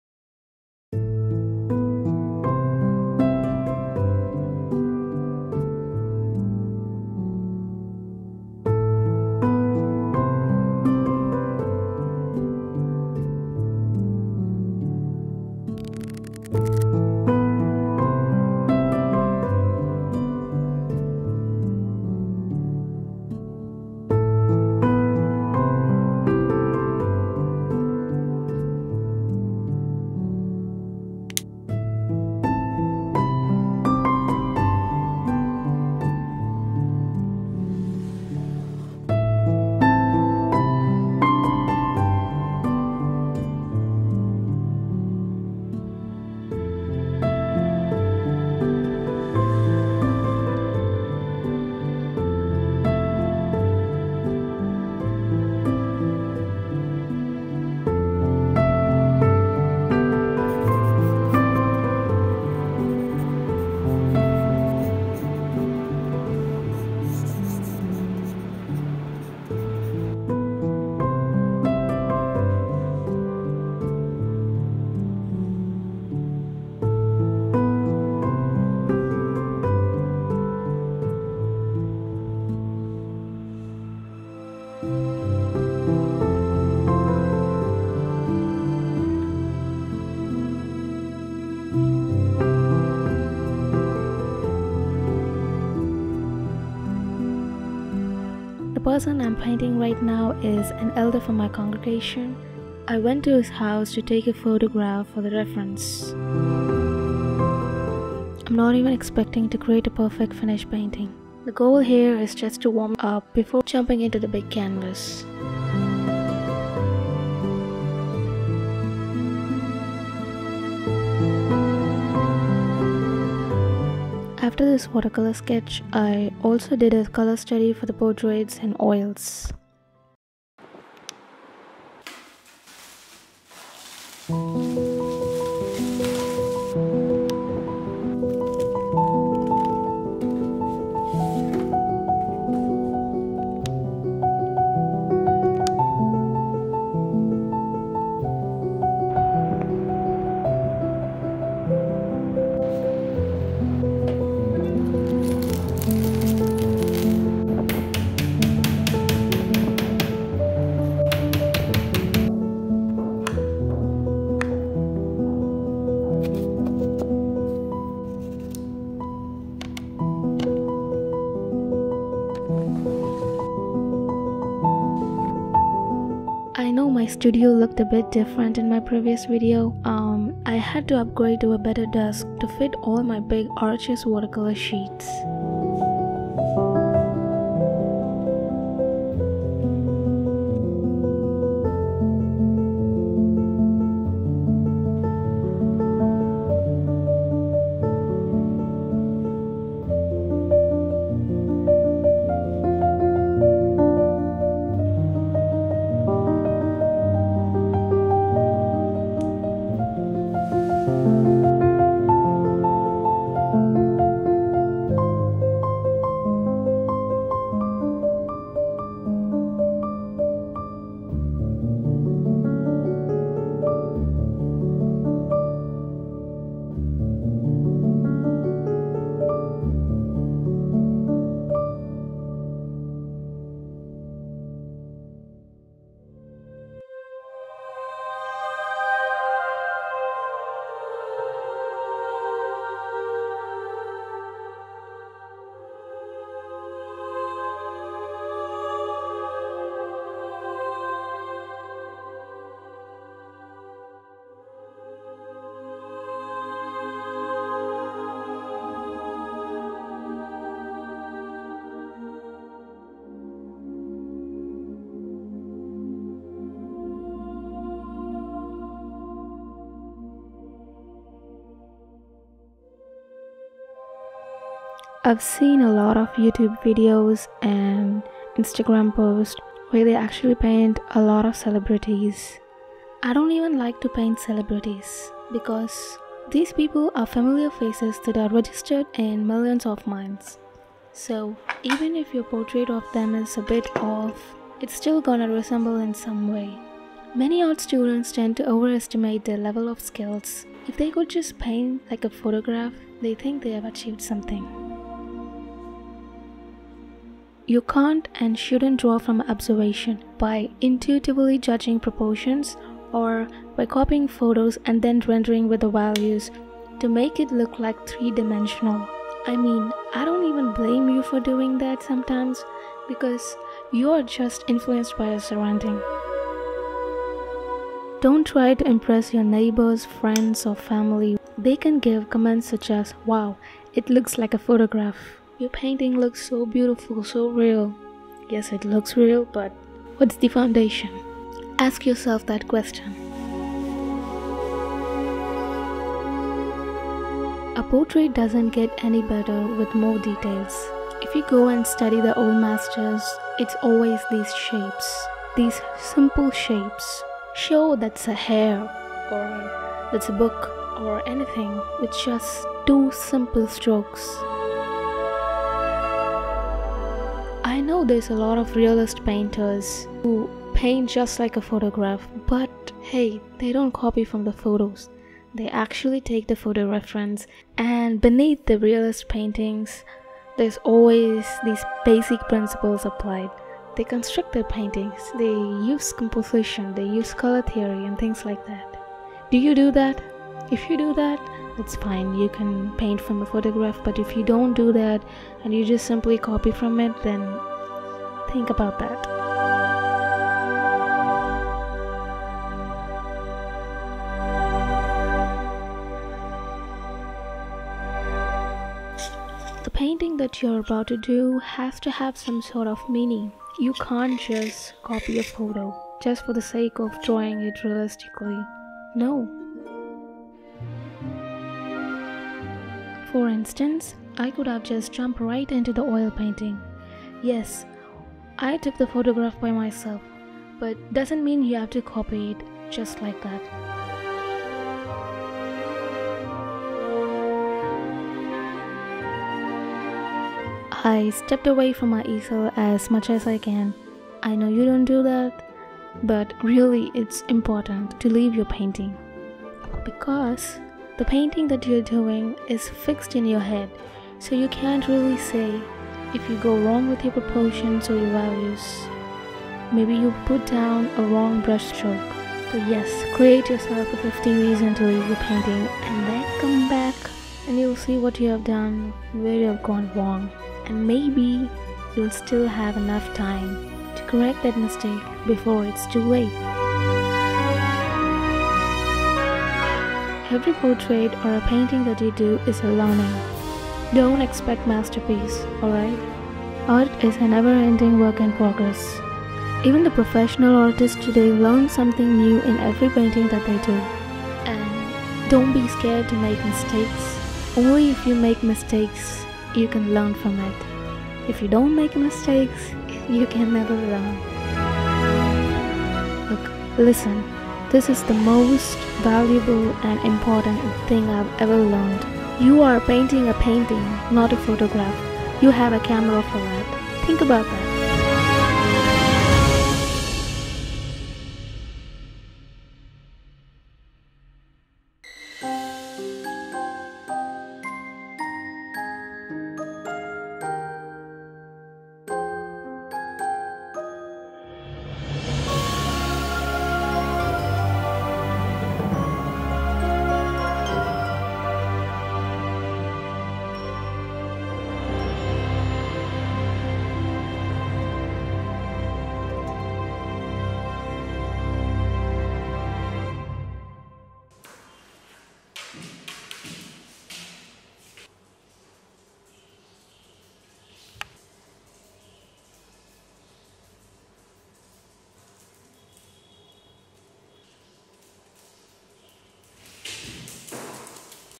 Learn from mistakes | cozy art vlog | creating a large oil painting ✨ unboxing for the studio 💫
In this video i make a watercolor sketch before i get into big oil painting. And i will talk about the importance of avoiding to paint the celebrities and what you should learn from making mistakes .

( Use my link to get 30 days of free trail)

🎬 Video equipment:
-Editing program: Final cut pro X
-Thumbnails : Photoshop / Procreate

Art books
John singer sargent book
Sorolla: The Masterworks
Anders Zorn: Sweden's Master Painter

Timestamps
0:00-0:34  Morning Routine
0:37-2:07  Watercolor sketch
2:10-3:36   Unboxing and adding new things to studio
3:37-3:54  Cozy bed reveal under sun
3:57-4:30  Applying gesso
4:31-8:42  Oil painting
8:43-9:13  Painting reveal
9:14-9:31  End credits (behind the scene)